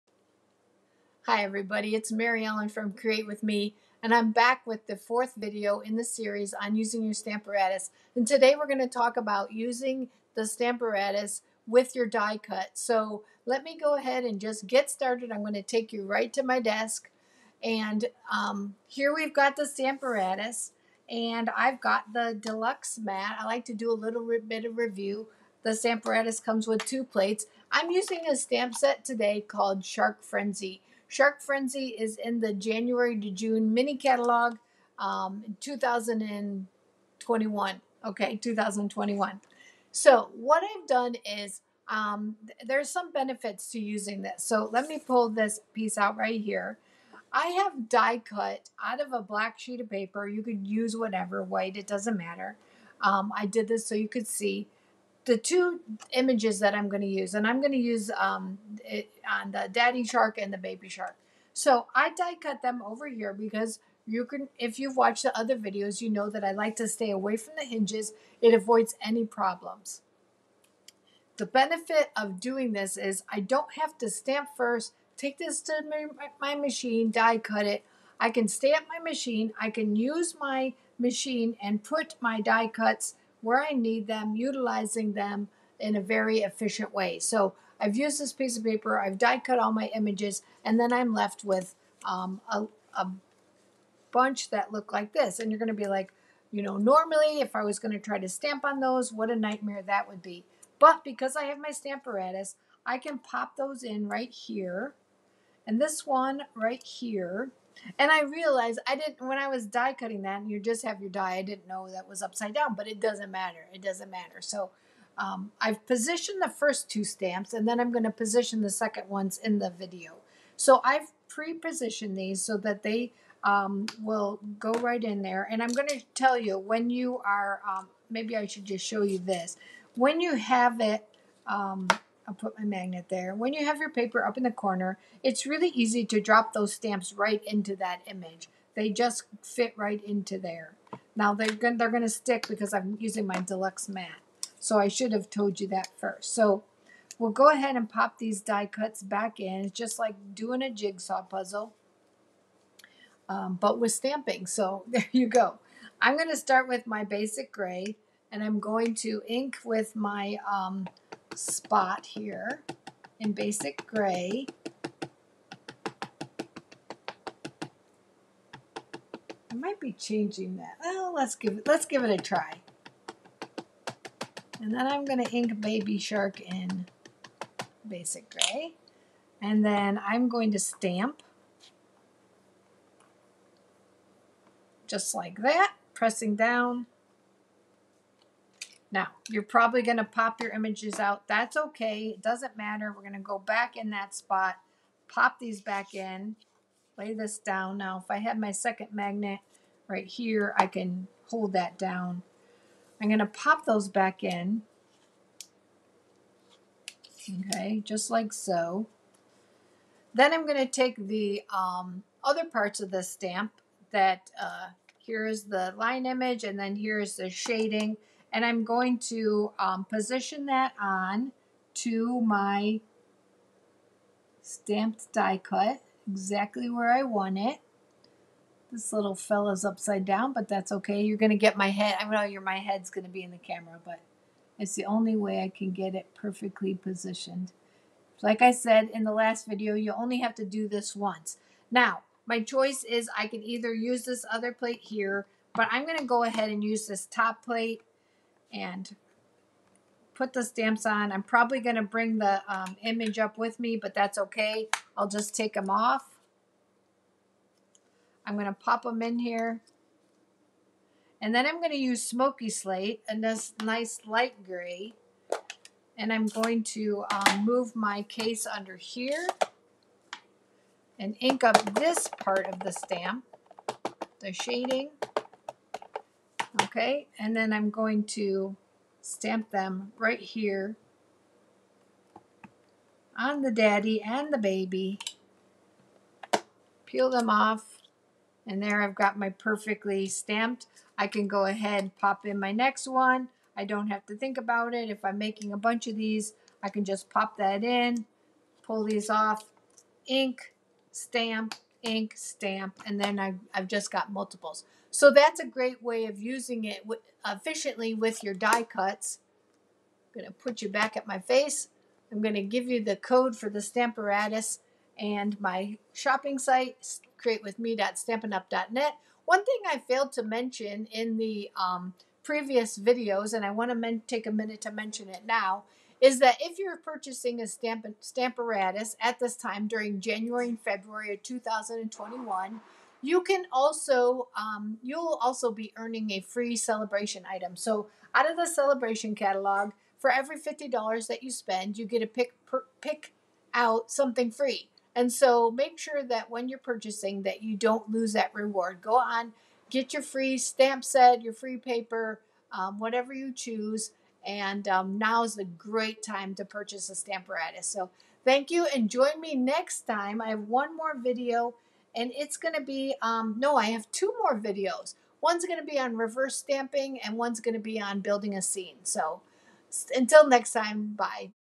Teach M.E. Tuesday Bonus Video Episode 4 Stampin' Up!'s Stamparatus Using it with your die cuts.
I have a 6 video series highlighting the Stamparatus and it's uses.  Everything from the basics to reverse stamping and hinge step stamping.  Learn how to use the valuable tool in your stamping.  Each Video is linked in my playlist and will be released weekly.

createwithme.stampinup.net